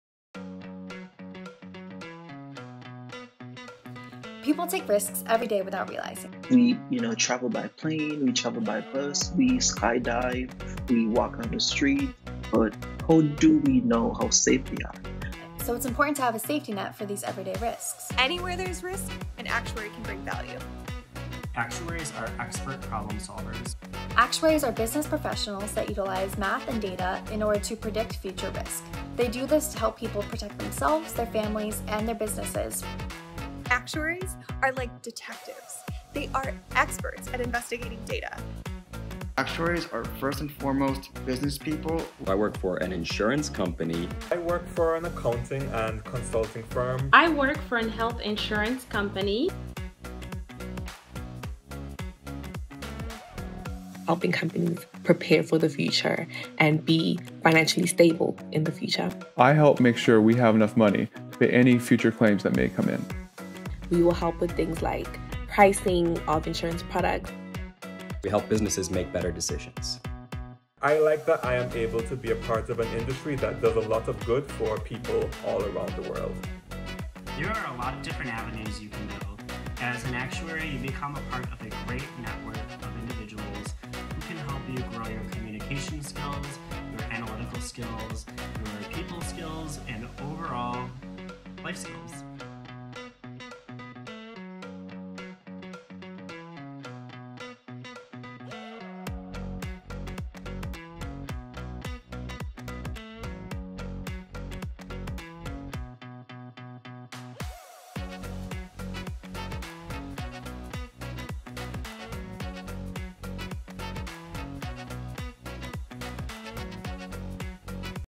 STEM Stars Actuarial Scholars Program: What Is an Actuary?
STEM Stars is a new scholarship program from The Actuarial Foundation and its Founding Partner, the Society of Actuaries, and other corporate supporters. The STEM Stars Actuarial Scholars Program identifies, recruits and supports skilled high school students interested in the field. We believe we can finally move the needle by providing help that starts in high school, extends into college and continues through a career.

The new STEM Stars Actuarial Scholars Program offers high school seniors a scholarship of $20,000 ($5,000 annually for four years). Along with financial support, it includes mentoring, tutoring, immersion experiences and guaranteed internships. It will be piloted at high schools where the Foundation has strong existing relationships. The first 20 scholarships will be awarded in Spring 2022. Learn more and apply at stemstarscholars.org.